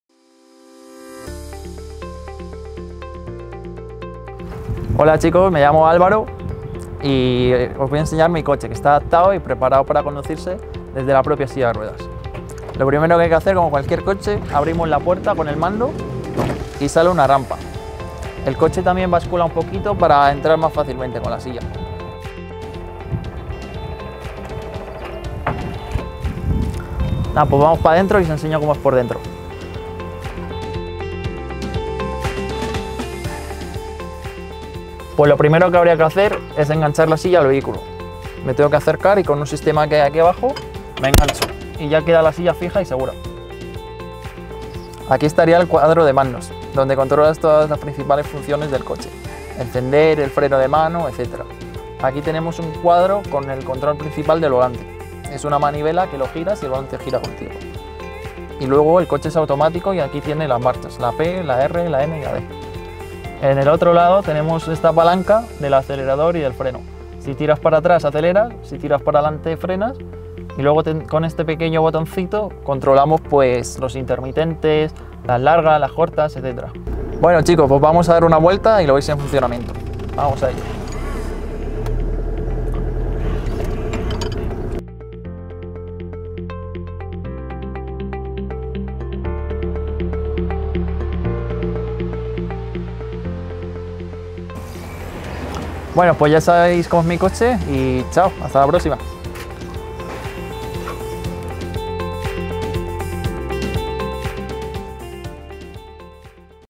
🚘👨‍🦽Conoces un COCHE ADAPTADO - Características y FUNCIONALIDADES 👩‍🦽
¿Quieres conocer cómo es el coche con el que Álvaro se mueve a diario sin limitaciones?